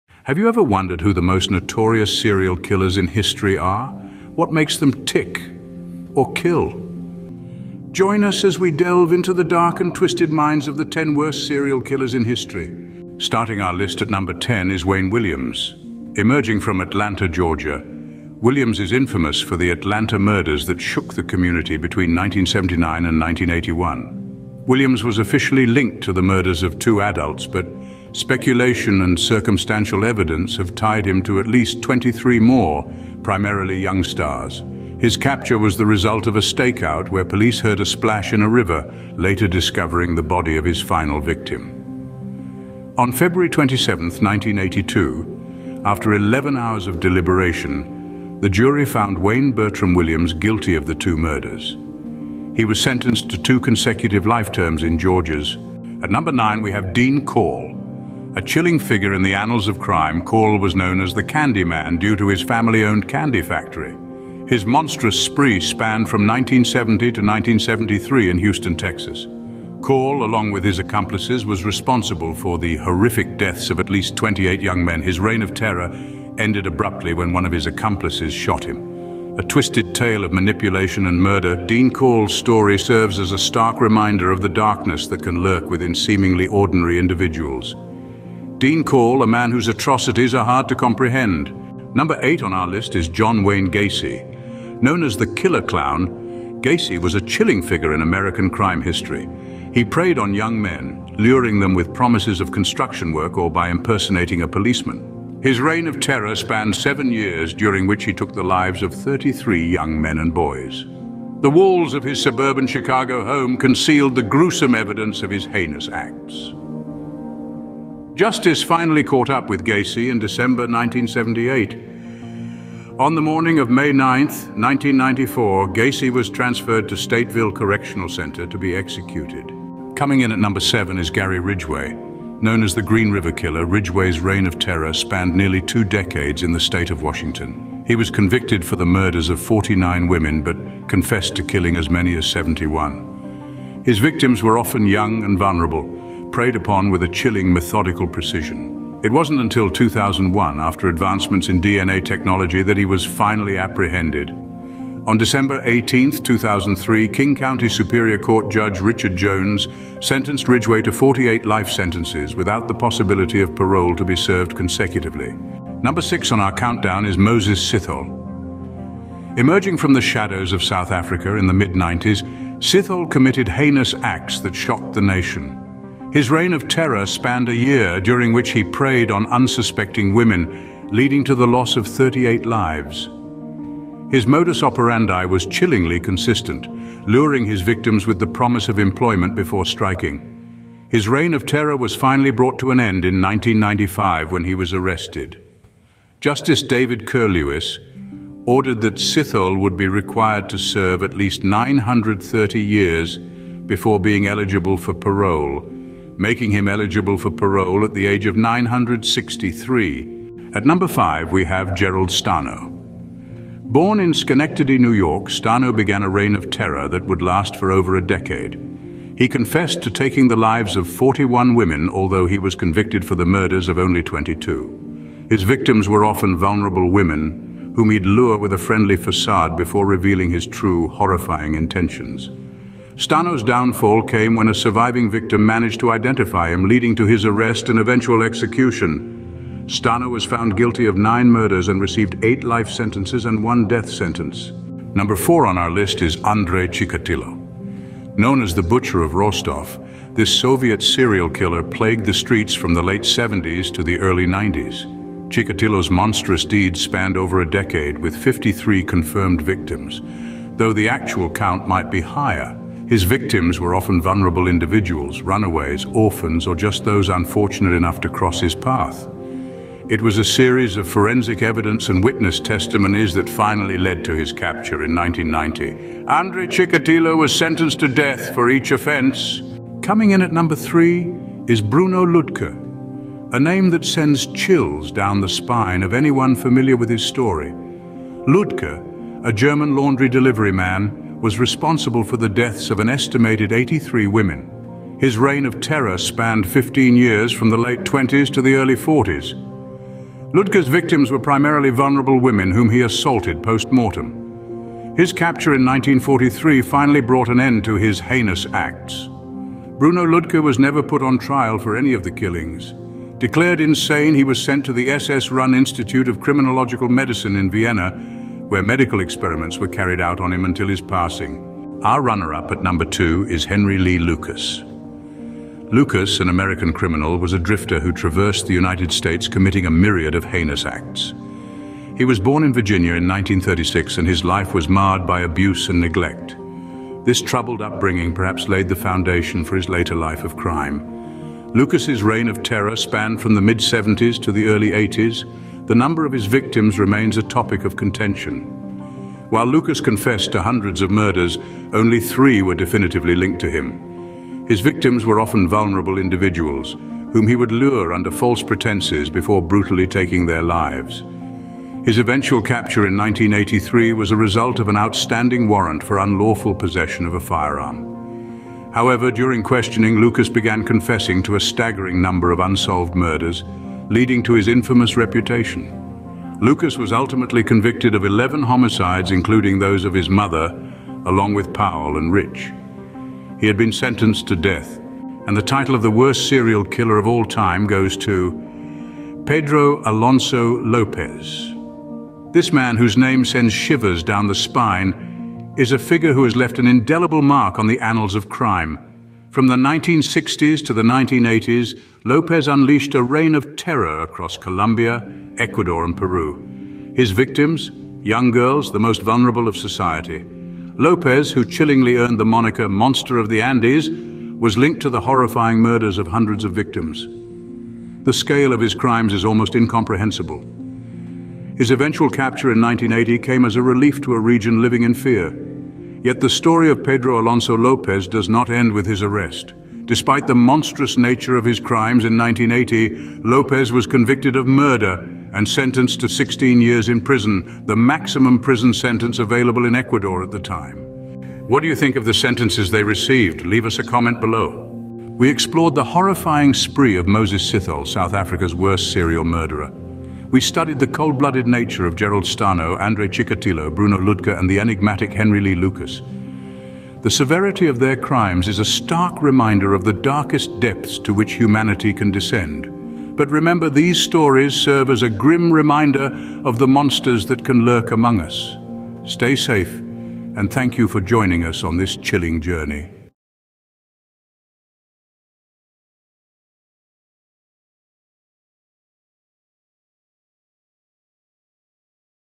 Worst Serial Killers In History : Most Dangerous Death Row Serial killers.
Top 10 most Dangerous serial killers in history. These are the worst death row inmates and death row execution list most wanted.

Join us on this spine-tingling journey that will leave you questioning about the minds of famous serial killers and when they were executed.

OUTLINE:

00:00:22  Wayne Williams
00:01:04  Dean Corll
00:01:40  John Wayne Gacy
00:02:18  Gary Ridgway
00:02:54  Moses Sithole
00:03:39  Gerald Stano
00:04:13  Andrei Chikatilo
00:04:48  Bruno Ludke
00:05:30  Henry Lee Lucas
00:06:34  Pedro Alonso Lopez

Crime | Crime scene: Crime scene investigation markers , Police investigating a crime scene , Forensic team at a crime scene